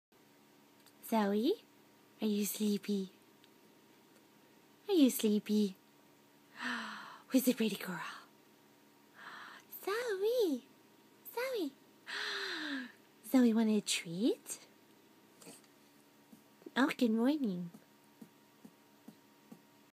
I only wake up for treats.
I only wake up for treats. (Don't worry, Mom gave me a delicious greenie after this!)